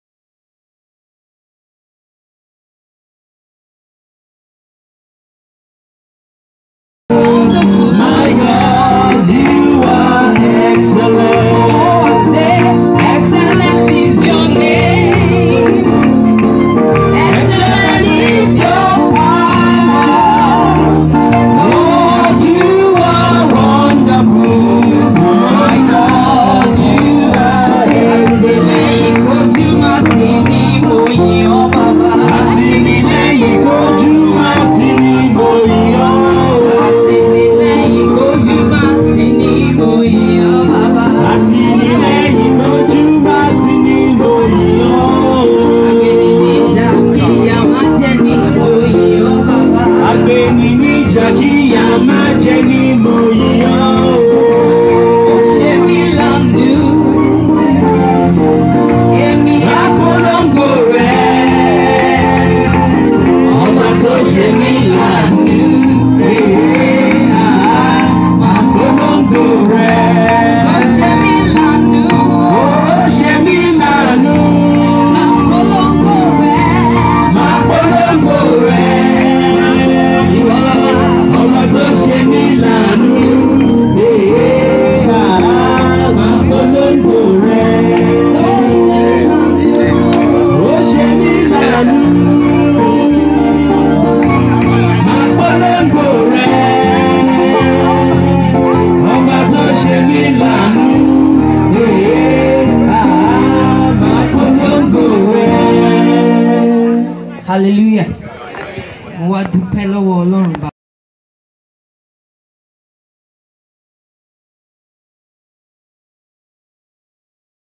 YEMI FEYI AARON SINGS@THEIR NAMING CEREMONY
IN APPRECIATION OF A COUPLE WHO IS DEDICATED TO THE WORK OF GOD AND HIS SERVICES